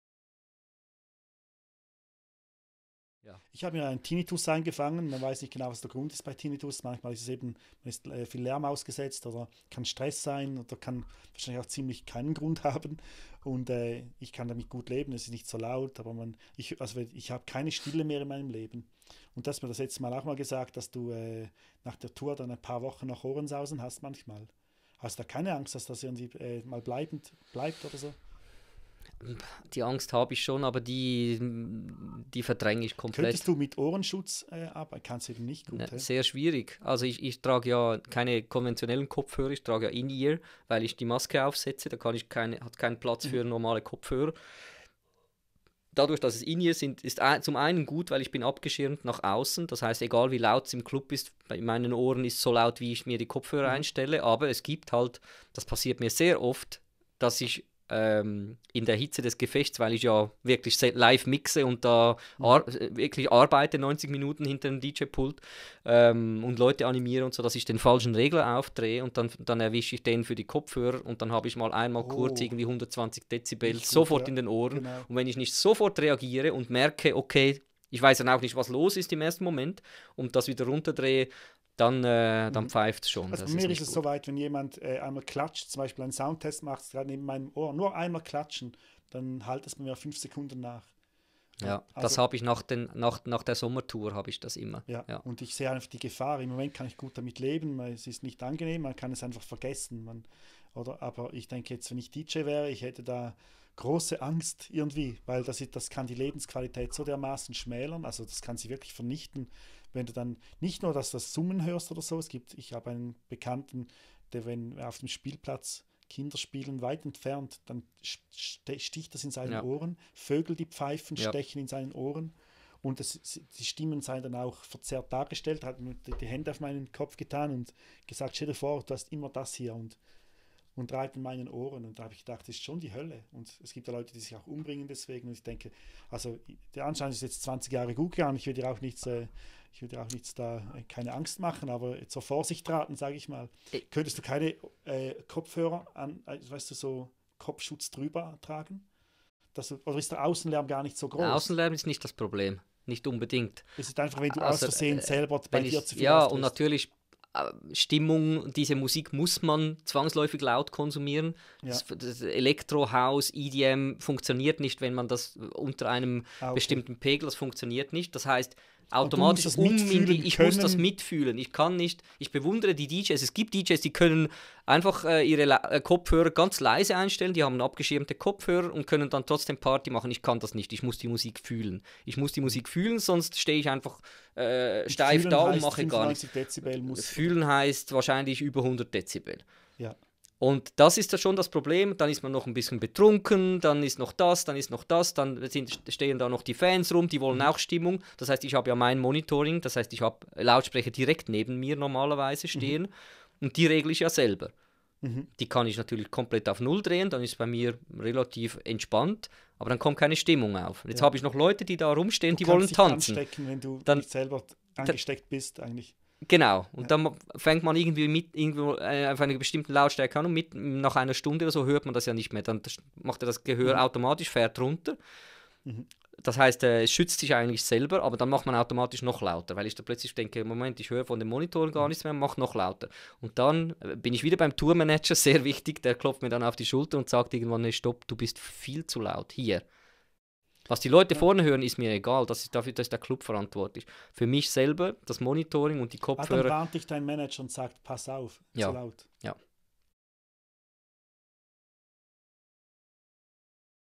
DJ Mike Candys über das Berufsrisiko Lautstärke
DJ Mike Candys spricht über das Berufsrisiko Lautstärke, Tinnitus & Gehörschäden